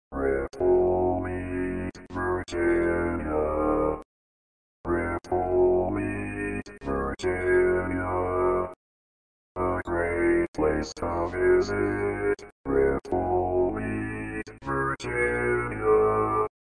How to Say or Pronounce USA Cities — Ripplemead, Virginia
This video shows you how to say or pronounce Ripplemead, Virginia.

A computer said Ripplemead, Virginia. How would you say Ripplemead, Virginia?